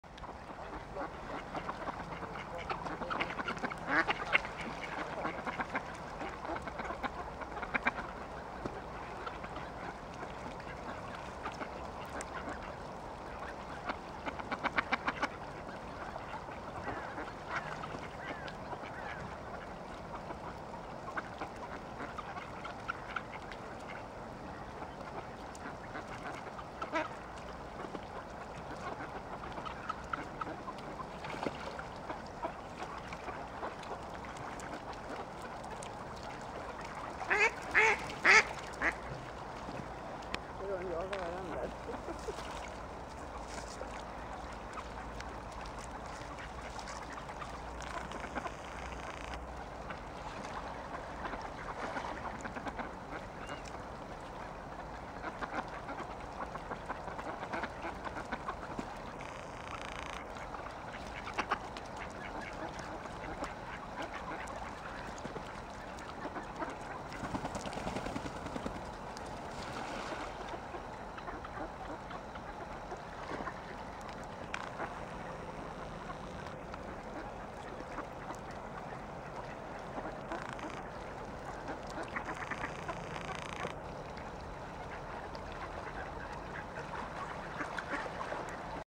Feeding the ducks.
Matar fina änder i sjön.

Feeding beautiful ducks in the lake.